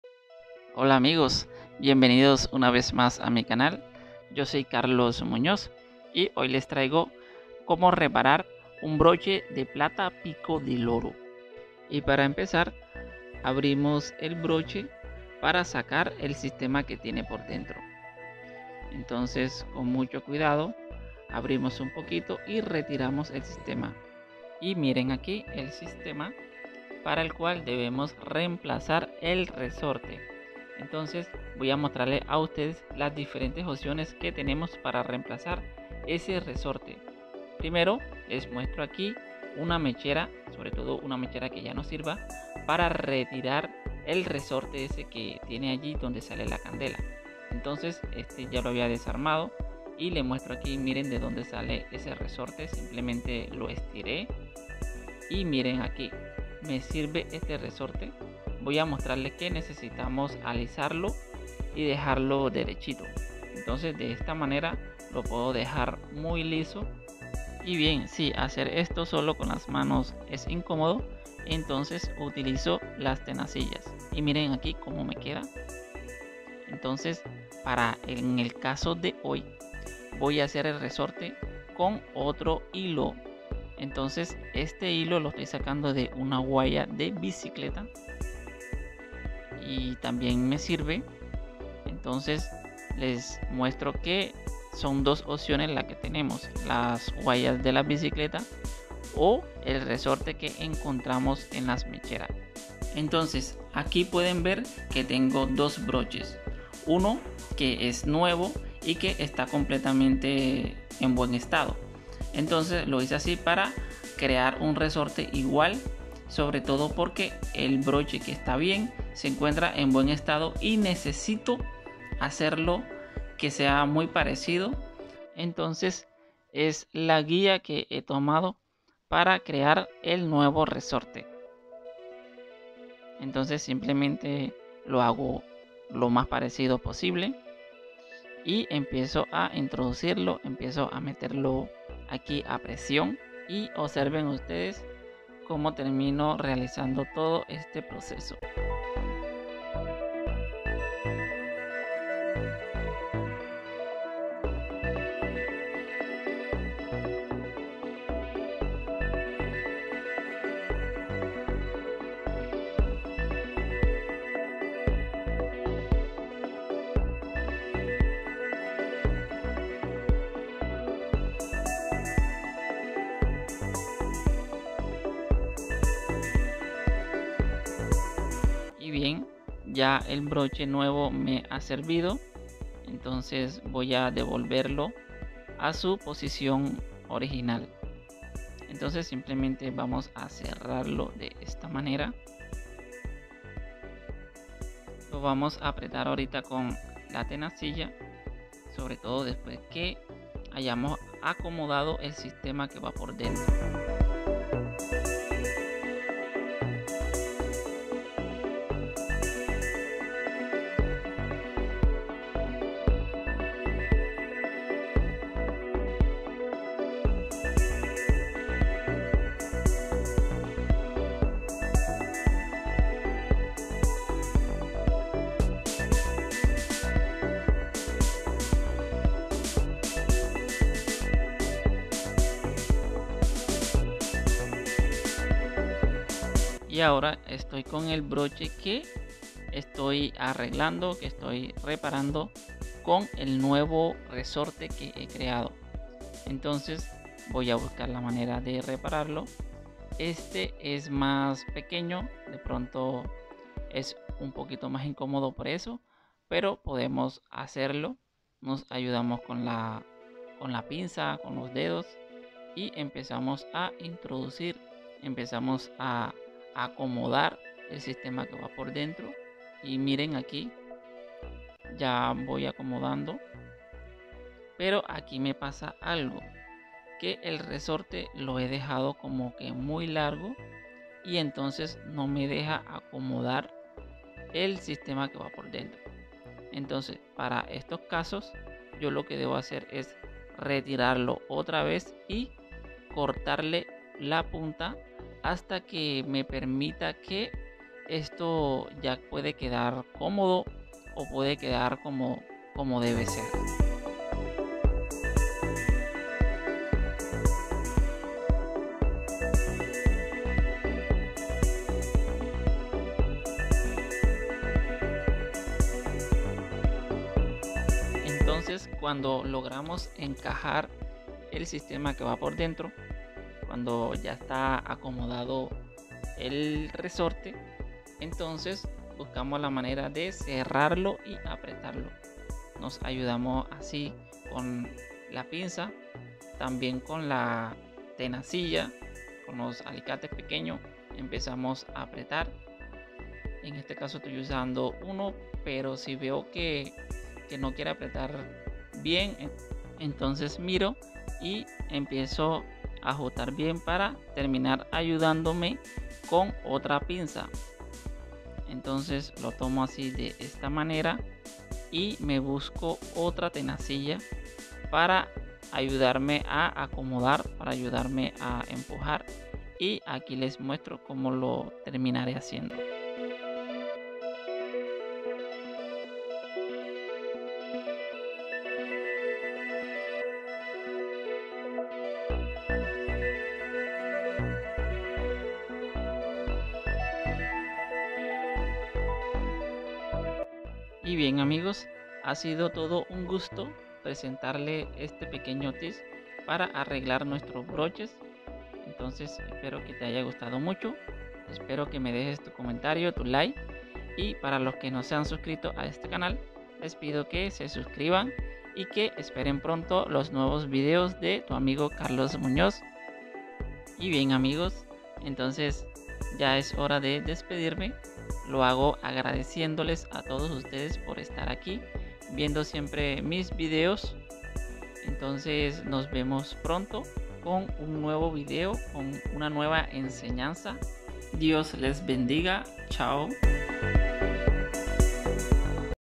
Reparación de broche pico de loro
Amigos la reparación la podemos hacer para los broches en plata o en oro, vale agregar que no se necesitan herramientas especiales y que por lo tanto pueden hacerlo en casa.